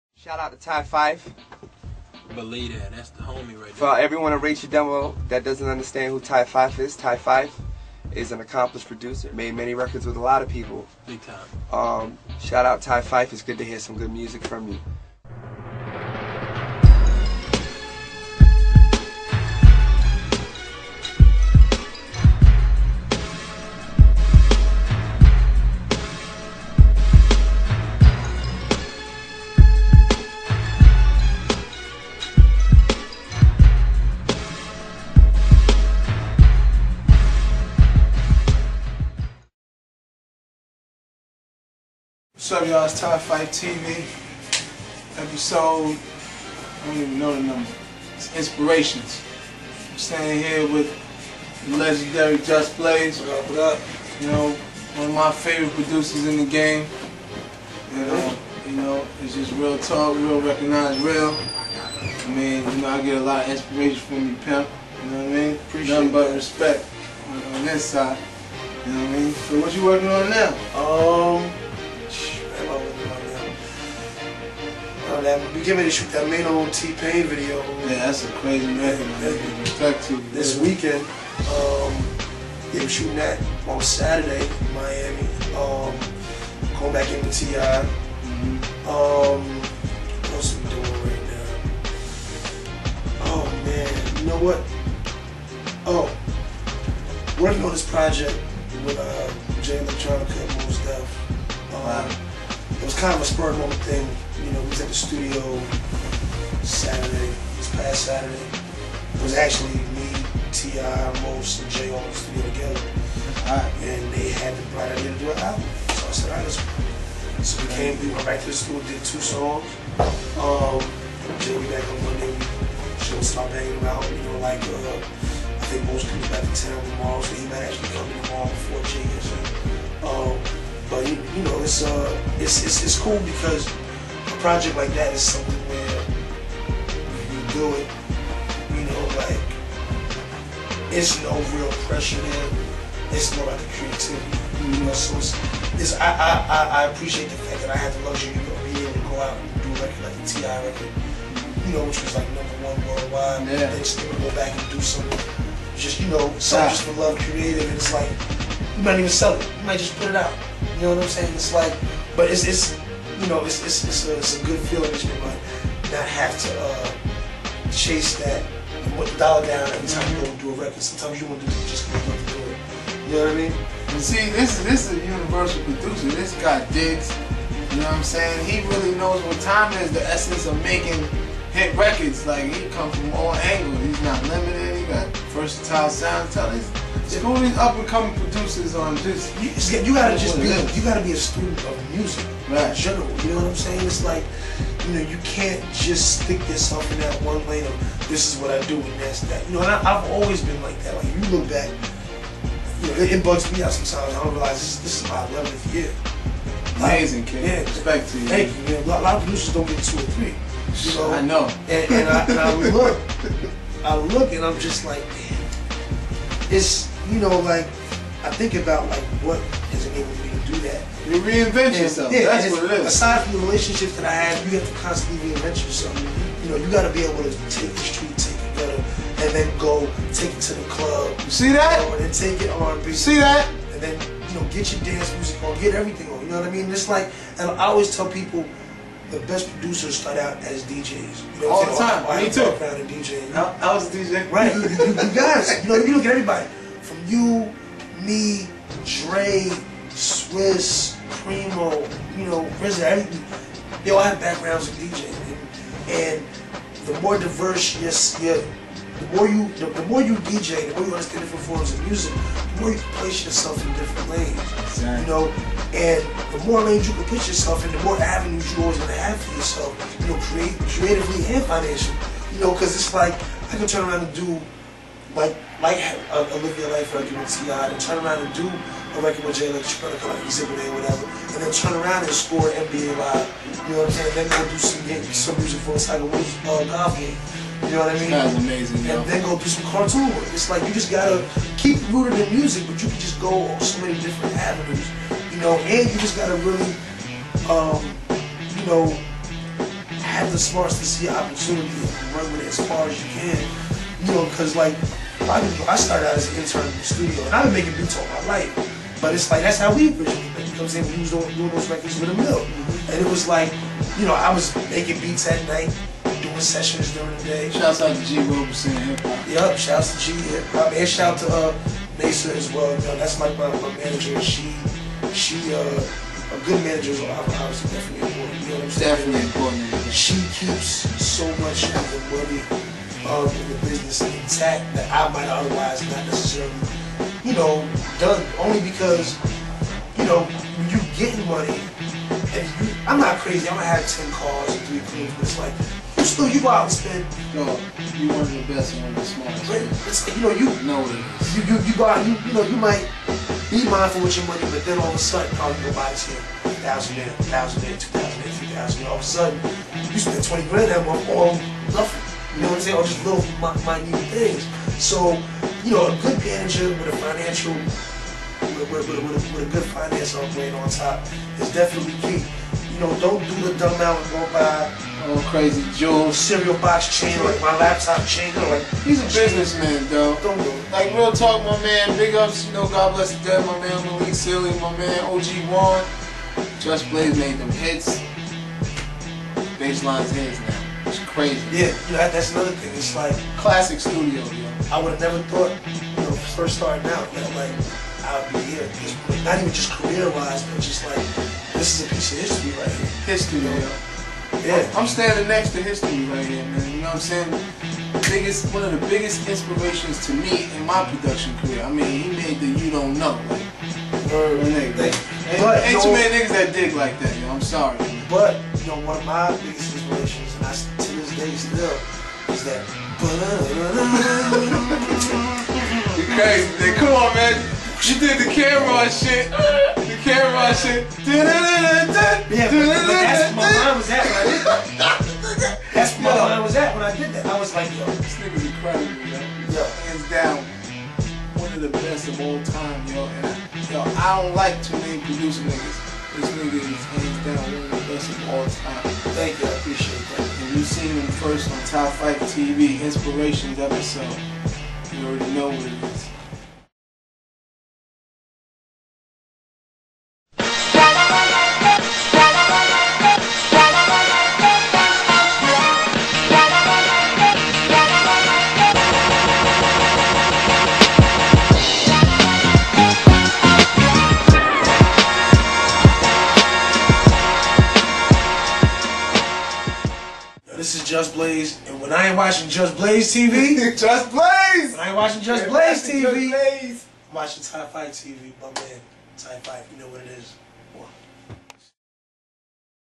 Episode 17 Ty Fyffe - The Inspiration
Ty Fyffe talks to super producer Just Blaze about future projects and how to be successful in music industry.